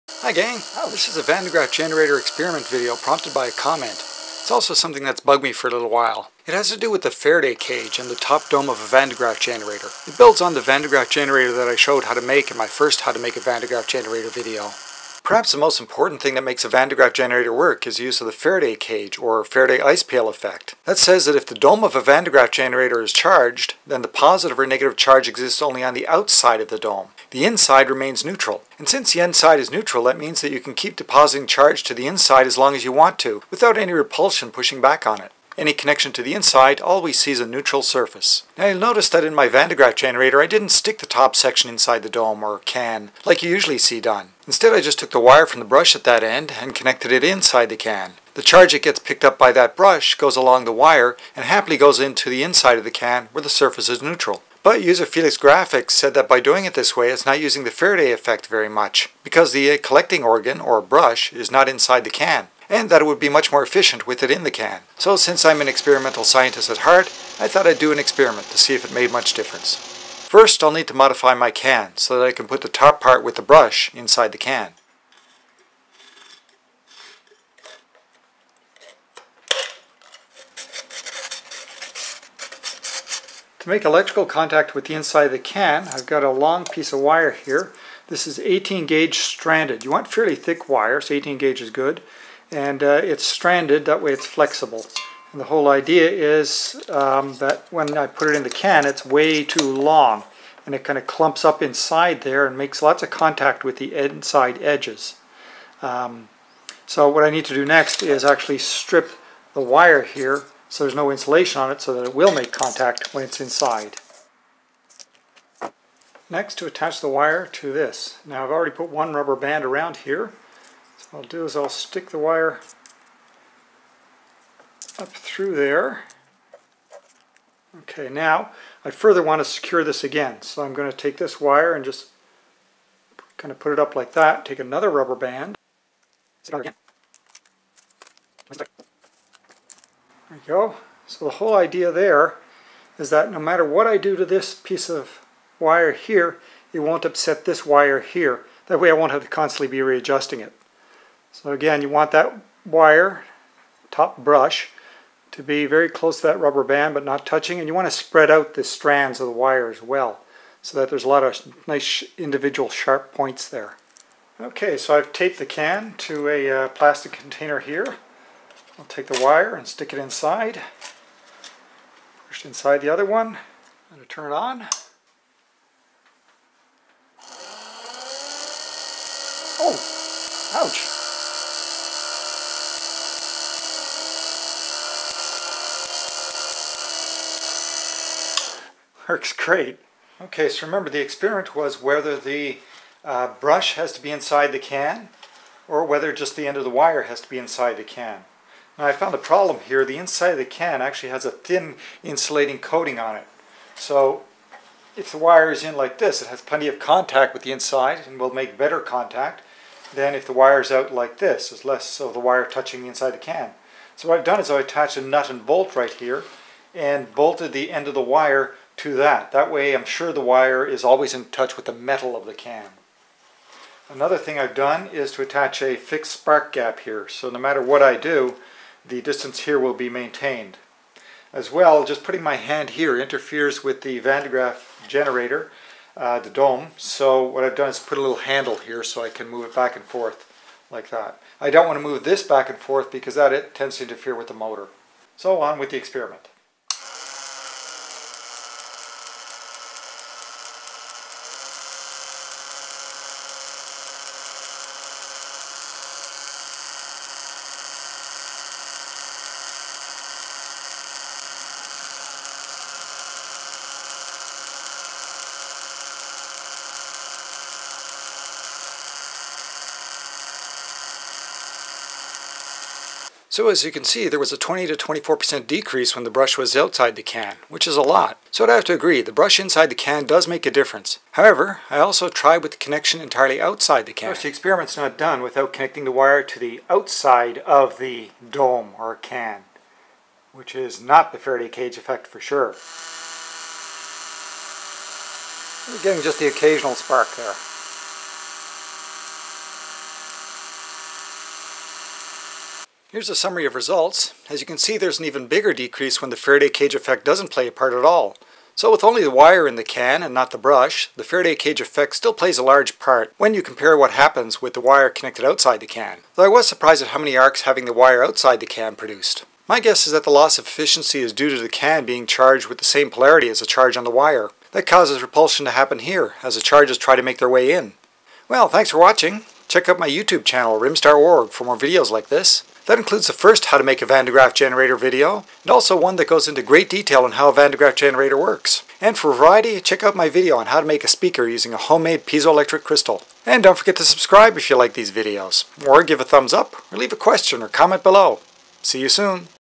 Van de Graaff Faraday Cage Science Experiment
Van de Graaff generator experiment to test the faraday cage effect of the Van de Graaff dome. In my first How to Make a Van de Graaff Generator video I didn't put the whole top part of the VDG inside the dome, as is normally done. Instead I just ran the wire from the brush into the dome. A comment from a user prompted me to do this rather scientifically done experiment to see if it makes any difference.

This video also builds on the Van de Graaff generator made in my first How to Make video and shows step-by-step how to make an improved one. It also helps clarify what a Faraday cage, or Faraday ice pail, is.